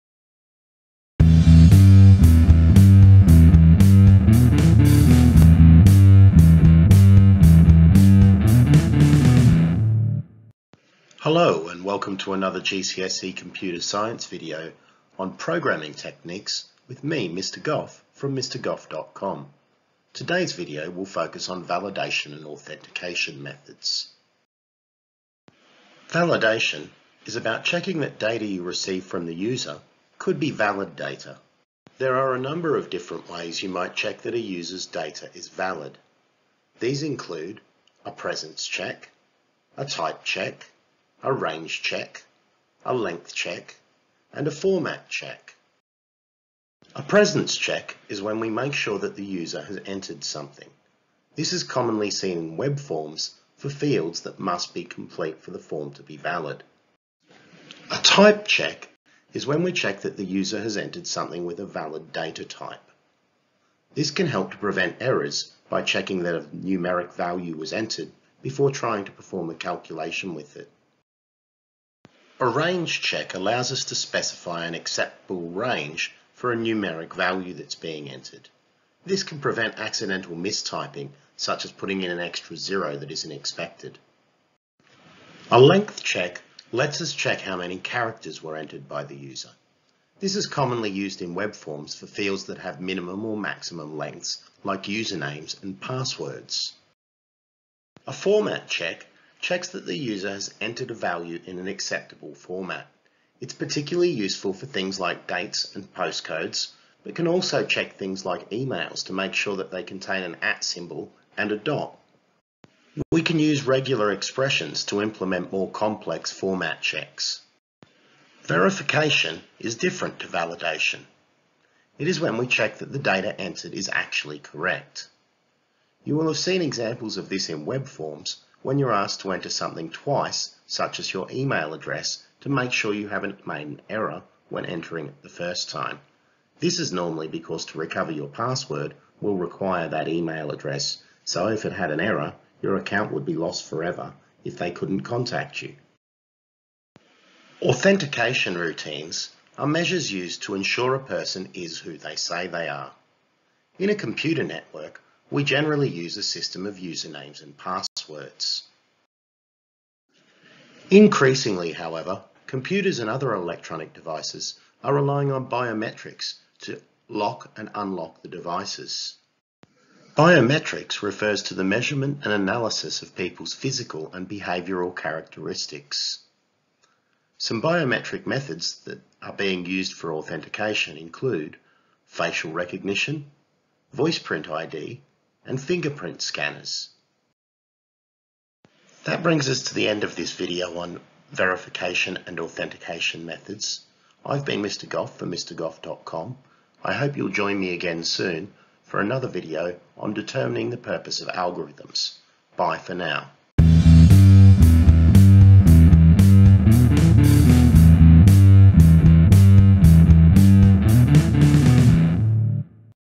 GCSE Computer Science: Validation and authentication
A GCSE Computer Science video in a series about programming techniques that is part of a larger series on GCSE Computer Science presented by Mr Goff from Mr Goff.com. This video focuses on validation and authentication methods.